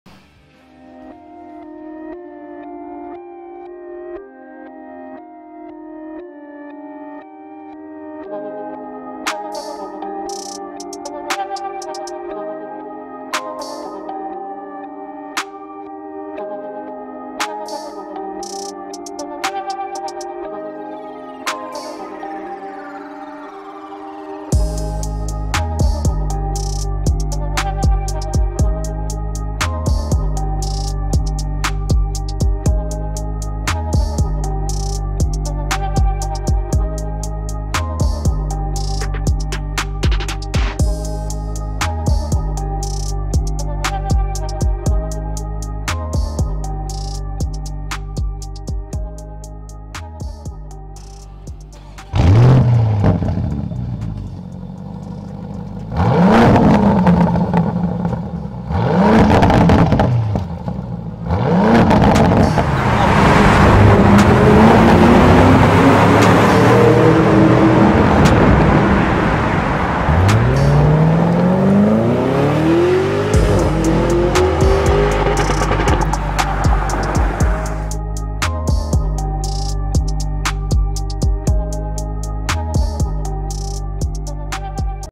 Lamborghini Urus SSUV Valvetronic Fi EXHAUST CatBack Install X Doctor Auto Clinic
Lamborghini Urus is fitted with Fi EXHAUST valvetronic catback and carbon fiber tips with gold inserts.

Thanks to automotive customization shop Doctor Auto Clinic (Thailand).

00:00-00:51 Intro
00:51-01:02 Idle Rev
01:02-01:09 Tunnel Run
01:09-01:16 Launch
01:16-01:25 Fi EXHAUST